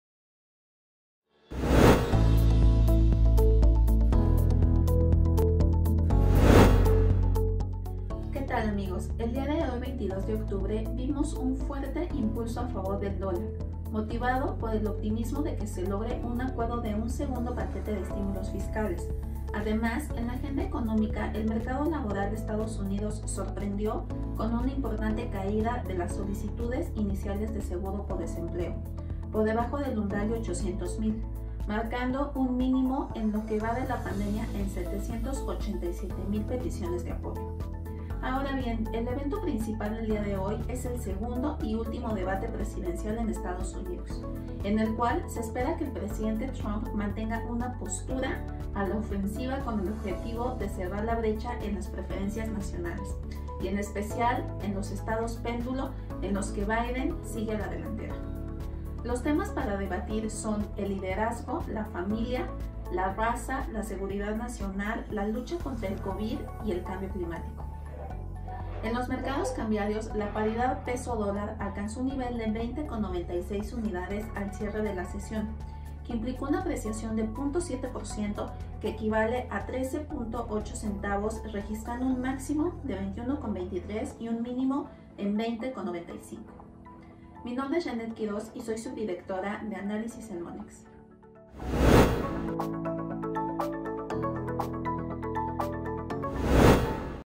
Reporte de Divisas - Cierre | 22 de octubre
Compartimos contigo el análisis de los últimos eventos en la sesión del día de hoy en los mercados internacionales de divisas por el equipo de Análisis Económico y Divisas | Monex